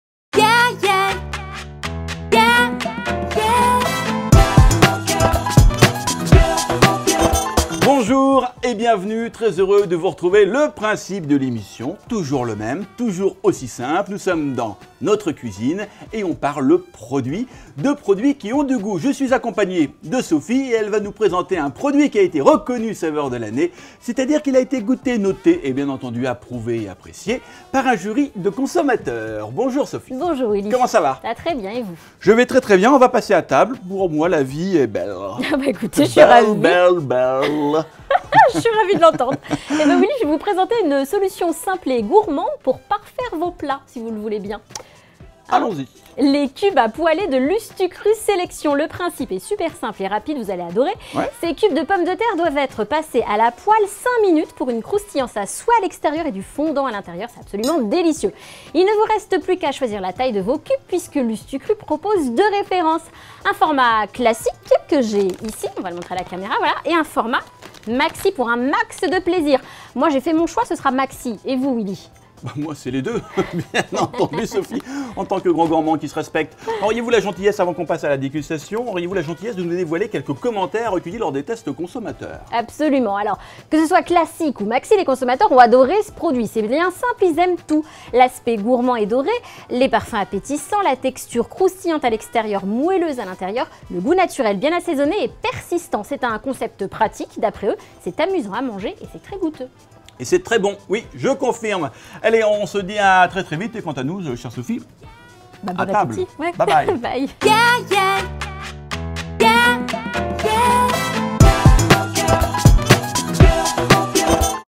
Cubes à poêler de Lustucru Sélection
Dans cette vidéo, découvrez de délicieux cubes à poêler à la pomme de terre, croustillants et fondants en seulement 5 mn de cuisson. Un format classique pour plus de croustillant. Un format maxi pour plus de moelleux et un max de plaisir !